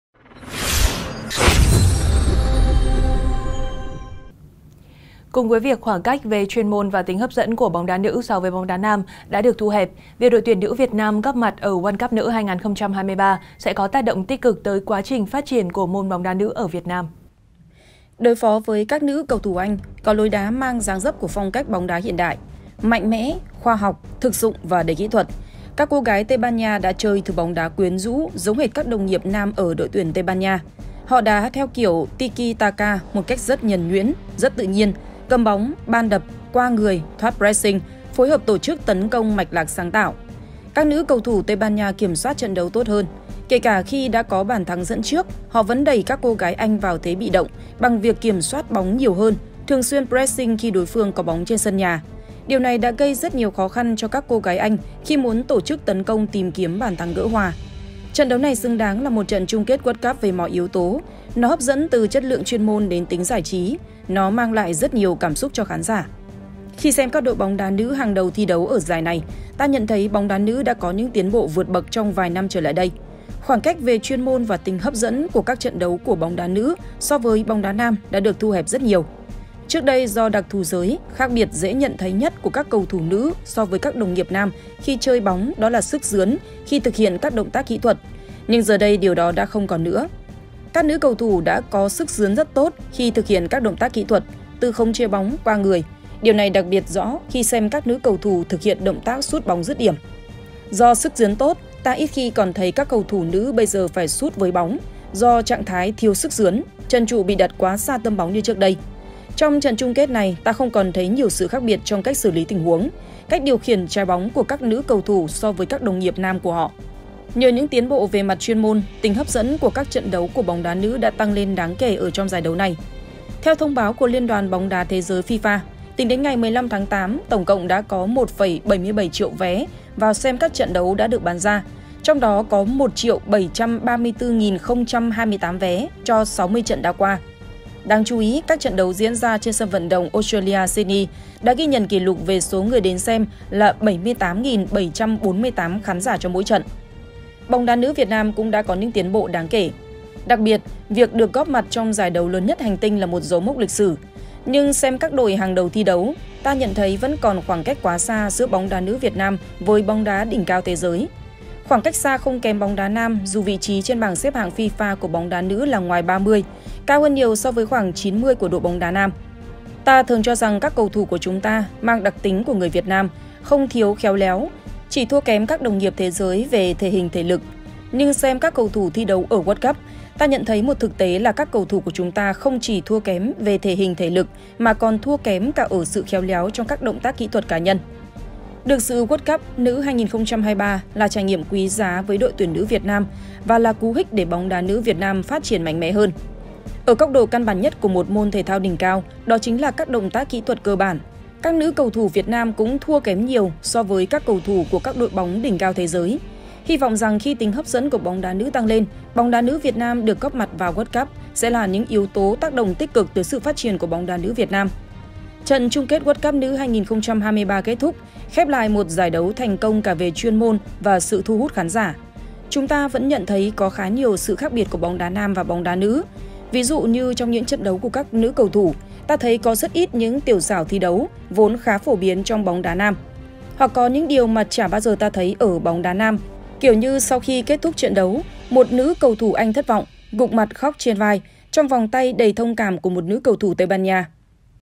World Cup nữ 2023. Bài học cho bóng đá nữ Việt Nam | Chiến Binh Sao Vàng
Chiến Binh Sao Vàng | Việc ĐT nữ Việt Nam góp mặt ở World Cup nữ 2023 sẽ có tác động tích cực tới quá trình phát triển của môn bóng đá nữ ở Việt Nam.

🕚 Những video hấp dẫn về bóng đá Việt Nam
©Bản quyền video thuộc về kênh Youtube CHIẾN BINH SAO VÀNG. Đề nghị không reup dưới mọi hình thức.